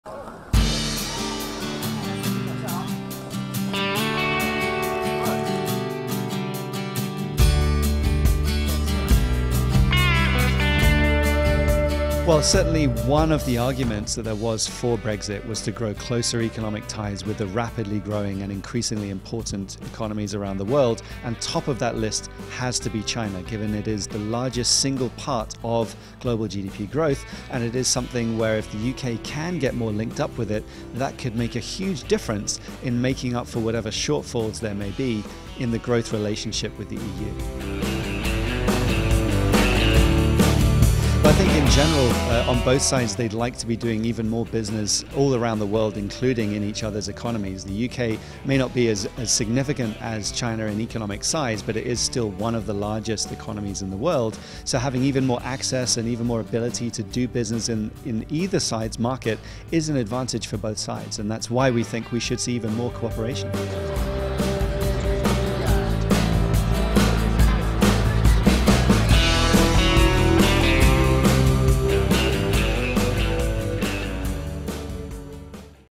Economist: China tops UK's economic partners list
UK is seeking economic partners and "top of that list has to be China." Standard Chartered Bank economist David Mann explains.